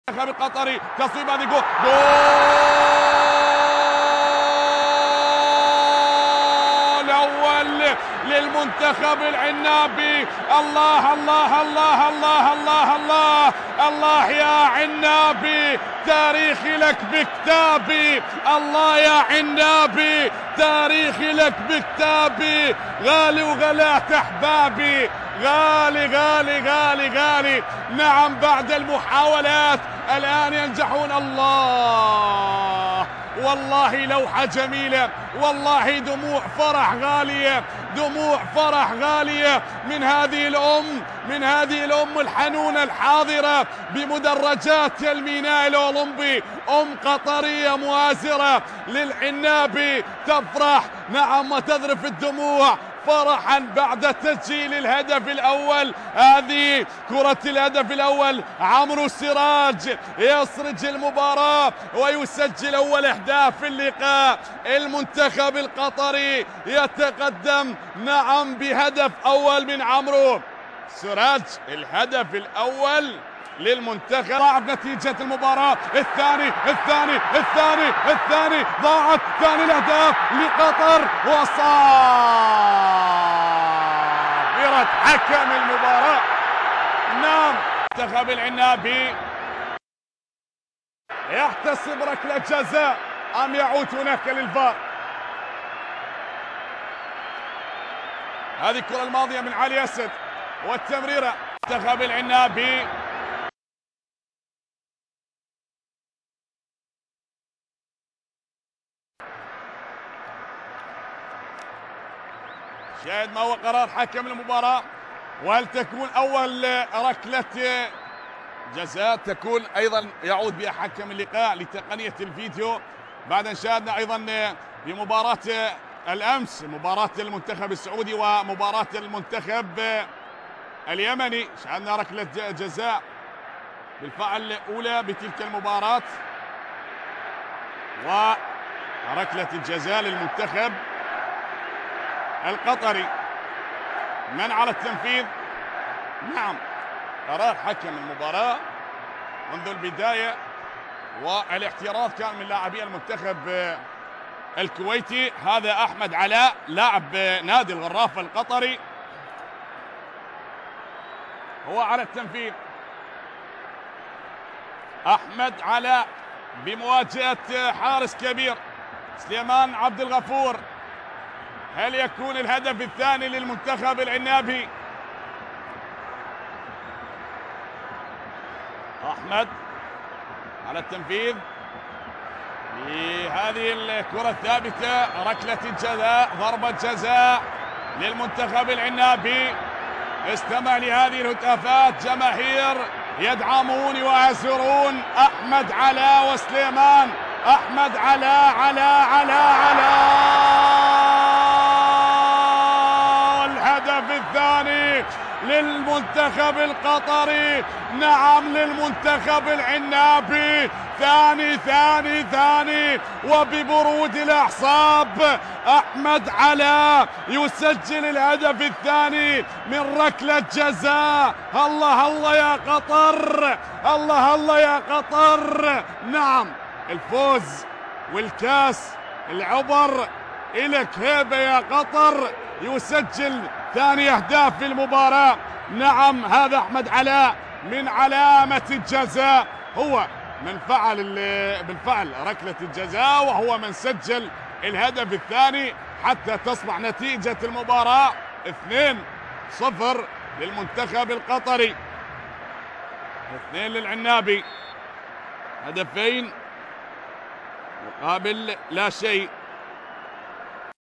اهداف مباراة منتخب قطر ومنتخب الكويت ضمن بطولة كاس الخليج العربي 25
منتخب قطر يفوز على منتخب الكويت 2-0 ضمن منافسات خليجي 25